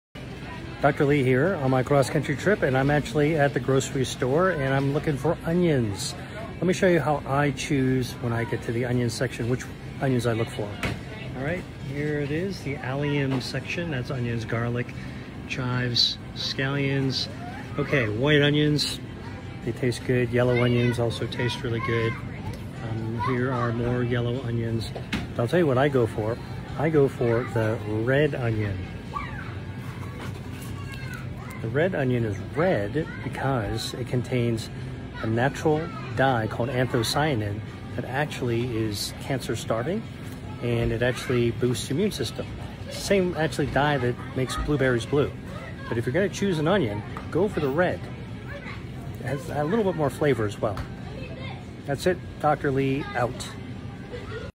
This Vegetable Can Reduce Risk of Heart Disease | Dr. William Li
All vegetables are important for eating to beat disease, but some veggies offer unique health benefits. Onions are one of my personal favorites! They contain lots of vitamins, minerals, and potent compounds to promote your health in many ways. Red onions are especially great because they contain anthocyanin. This is a plant pigment that gives the onion its unique color. Studies have found that people who consume more foods rich in anthocyanin have a reduced risk of heart disease. What's your favorite meal to make with red onions?